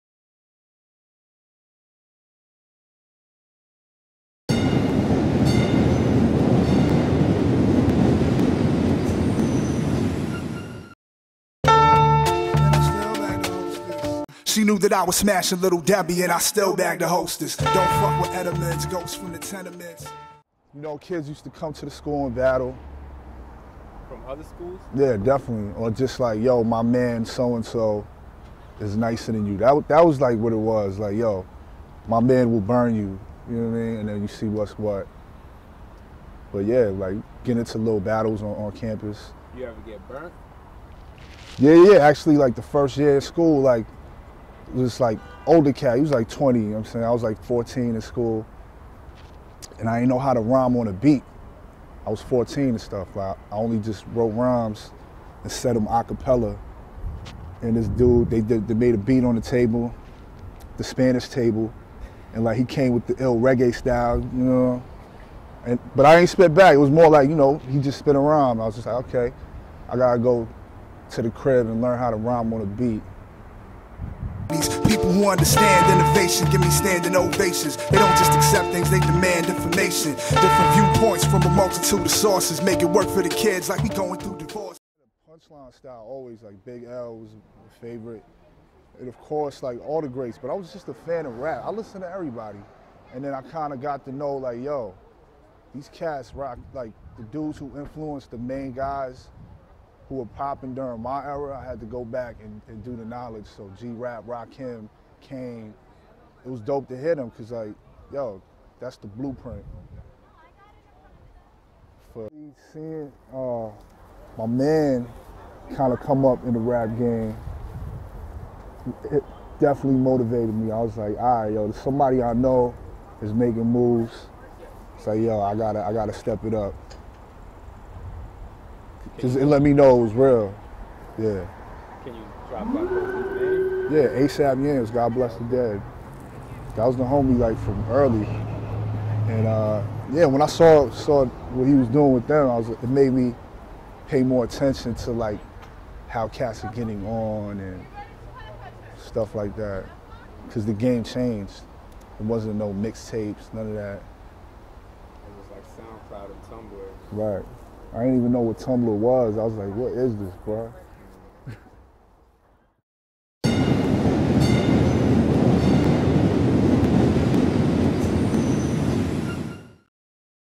FIELD TRIP E03: YOUR OLD DROOG ON W8th STREET (CONEY ISLAND)
@YourOldDroog plays The Ave like a true Brooklynite with the @wemadeitnyc crew in tow. Posted up outside where he took his GED courses (and smoked a blunt or two) he recalls his first rap battle, doing his research on rap's greats and how his friend A$AP Yams showed him the blueprint for getting on.